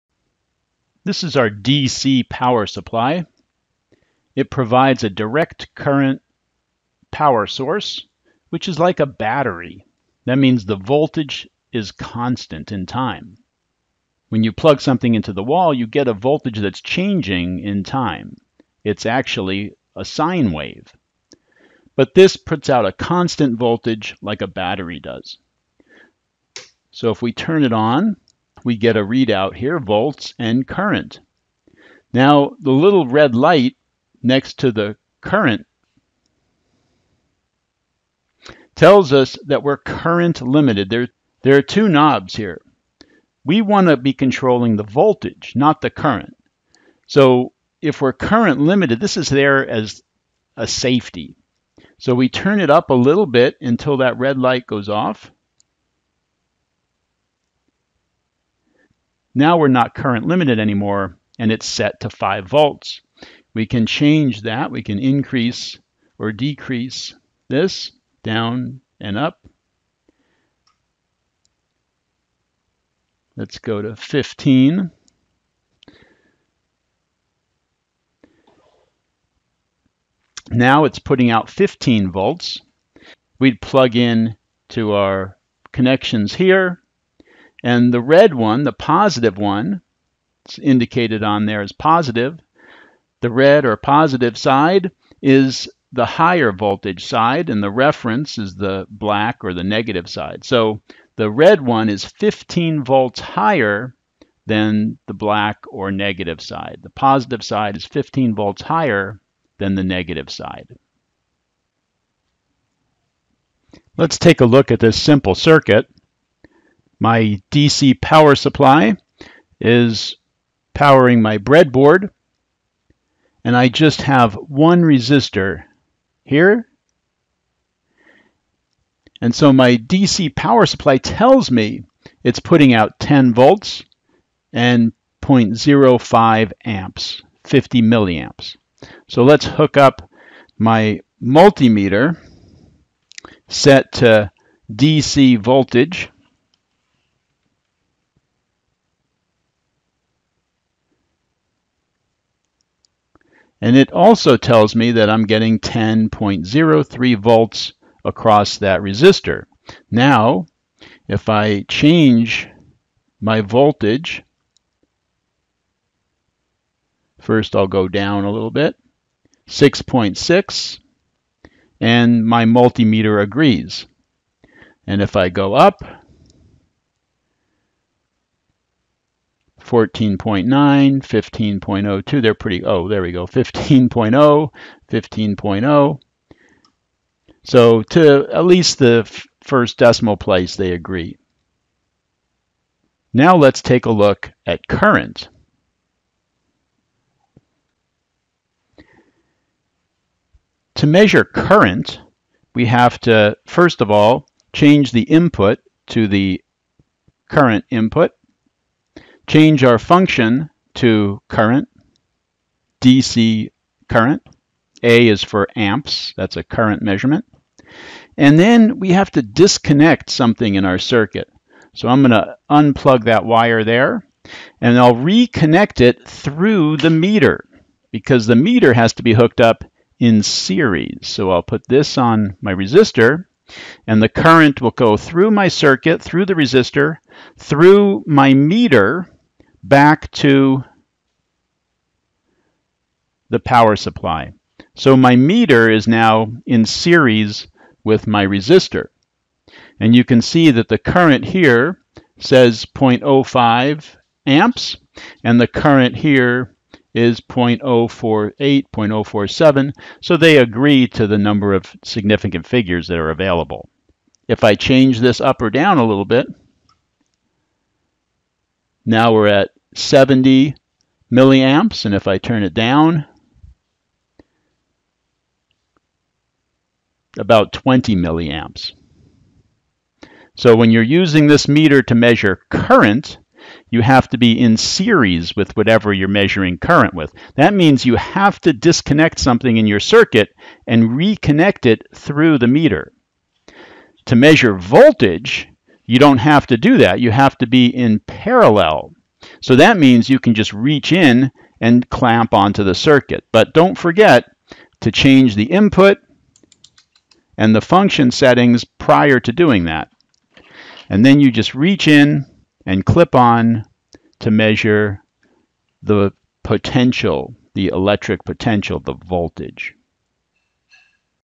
Lab Equipment: DC Power Supply
This video supports our Introduction to Equipment lab for electricity and magnetism.